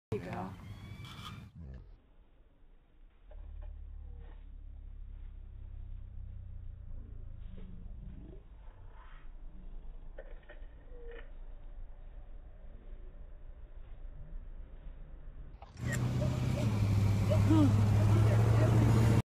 Bald American Eagles in San Juan island
This is a once in a life time event for me. I never imagine that I could see a bald eagle from that close and getting a piece of chicken from the ground.